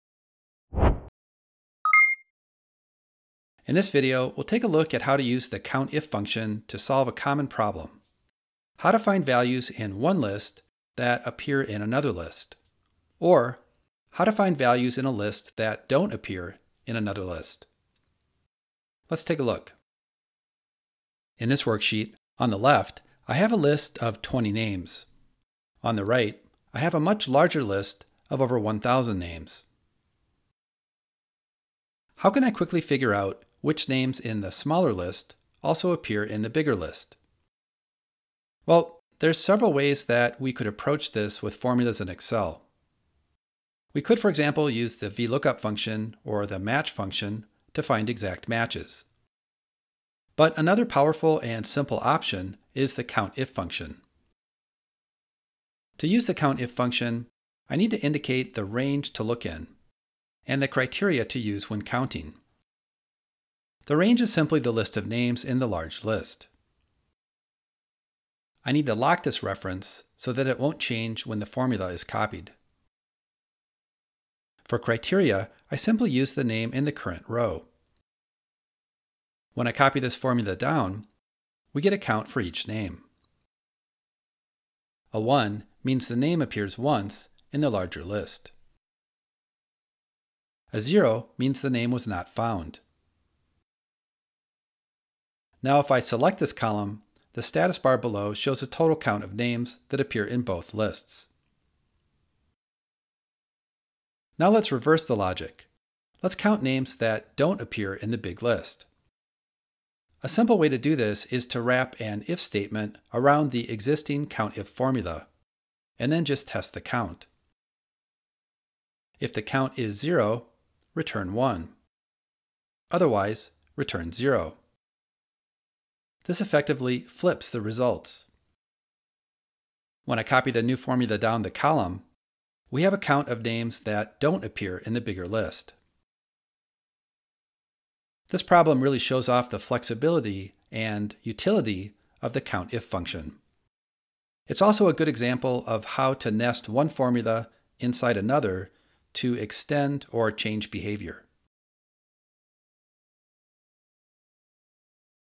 How to find missing values with COUNTIF
In this video, we take a look at how to use the COUNTIF function to solve a common problem: how to count values in one list that also appear in another list. We also "flip" the formula to count values in one list that are missing in another list.